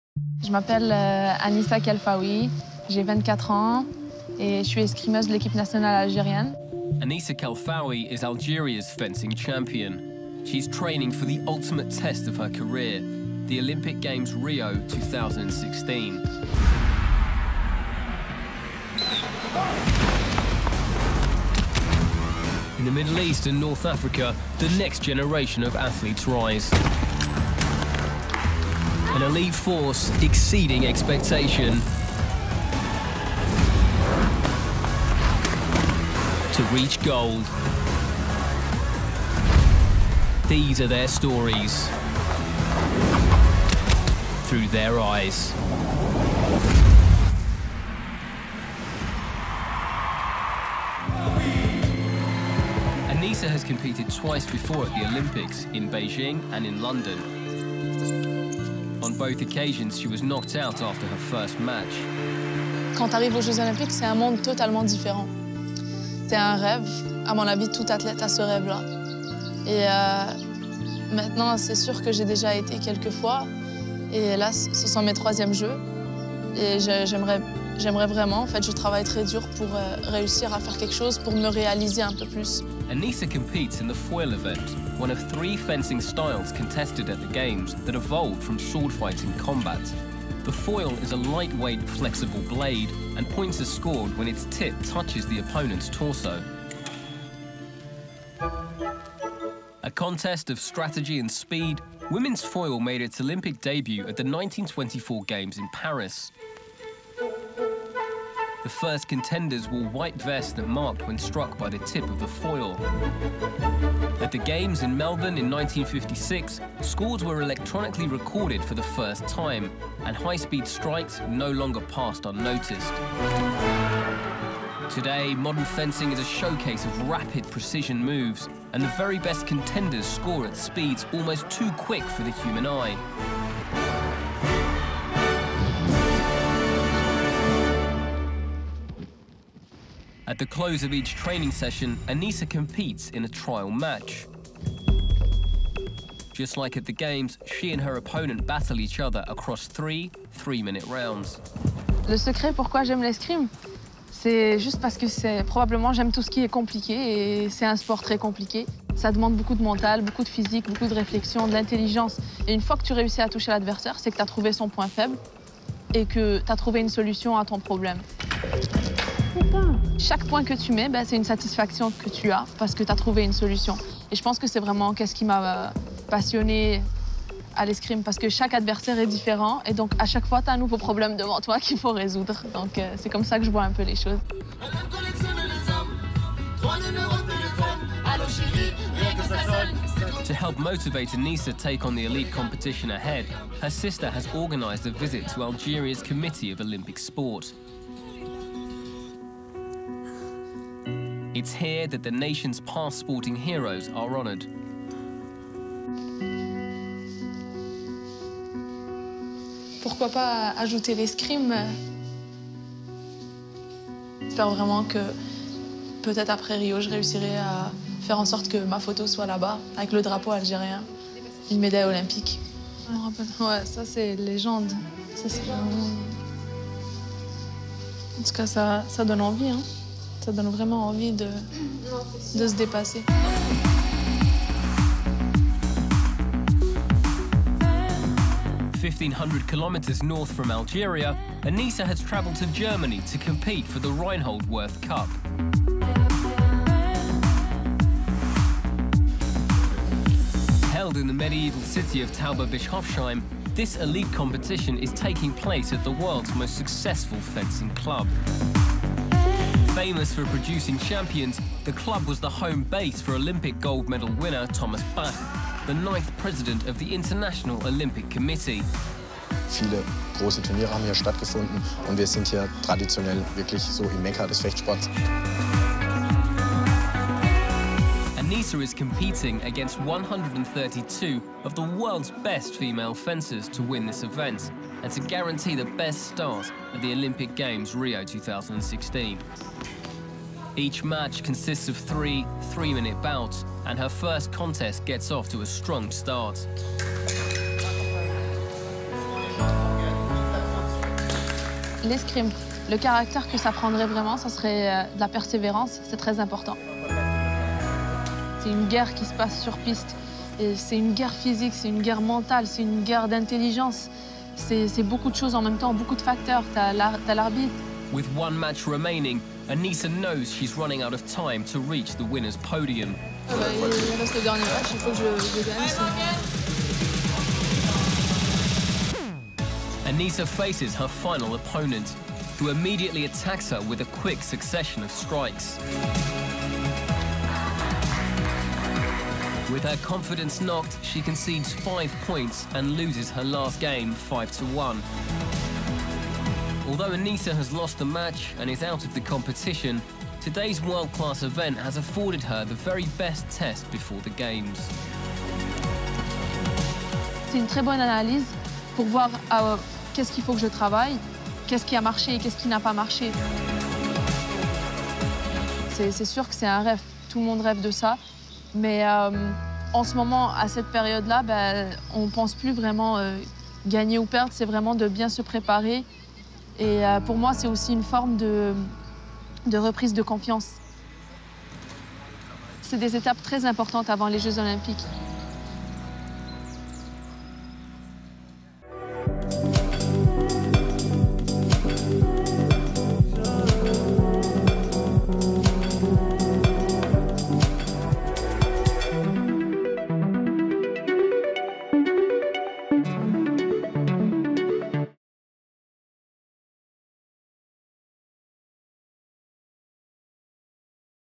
Algerian Olympic Legends Inspired Anissa Khelfaoui's Road to Rio | Generation Rise
Fencer Anissa Khelfaoui takes inspiration from legendary Algerian Olympians as she pushes herself in preparation for her third Olympics.